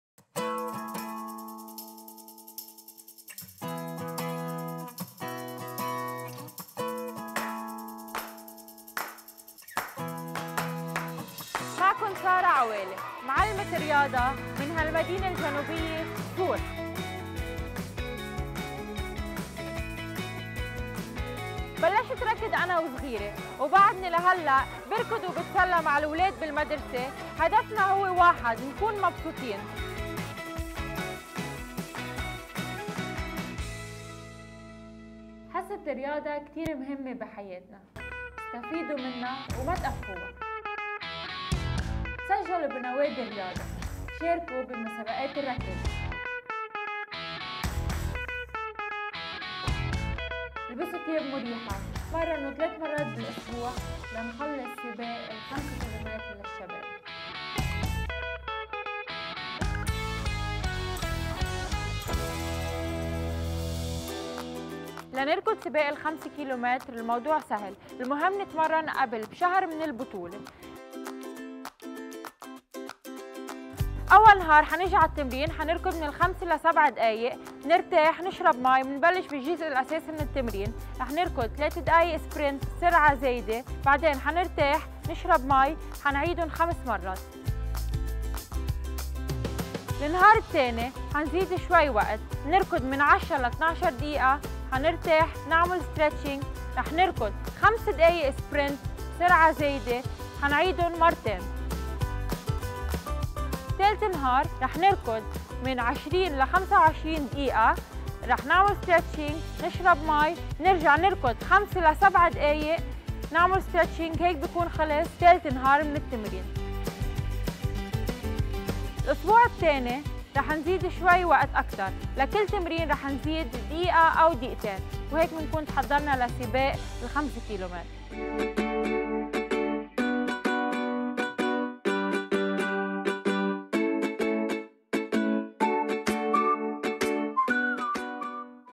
Youth Race Tutorials - Coach Sarah #1
Coach Sarah films her tutorial all the way in Sour and specializes in training 14-15 year olds for the BankMed Youth Race taking place on March 22!
It's a practical and easy guide to get ready to run the 5k race!
Organized by the Beirut Marathon Association